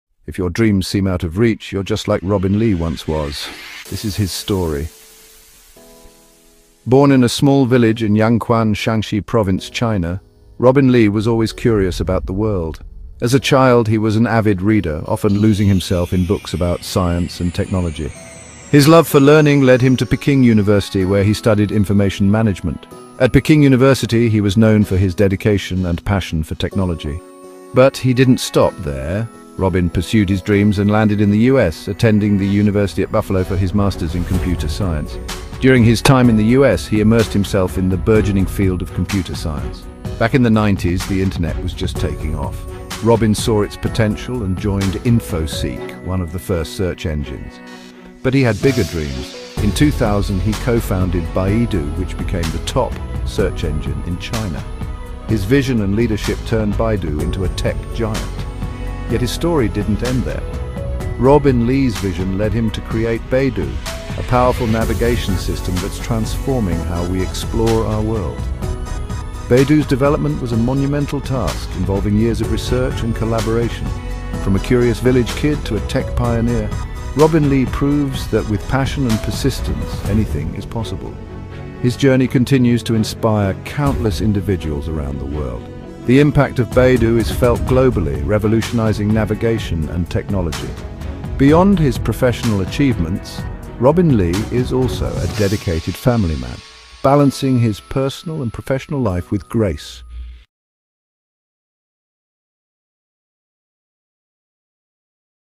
Who is Robin Li? The Journey of Beidou's Founder
Explore the inspiring journey of Robin Li, from his humble beginnings in a small village in China to studying at prestigious institutions like Peking University and the University at Buffalo. Learn about his remarkable career progression, from joining Infoseek to co-founding Baidu, China's leading search engine. Discover how Robin Li's innovative spirit led to the development of Beidou, a navigation system that is revolutionizing exploration. Don't miss this compelling story of ambition, perseverance, and groundbreaking achievements.

OUTLINE:

00:00:00 Who is Robin Li? The Journey of Beidou's Founder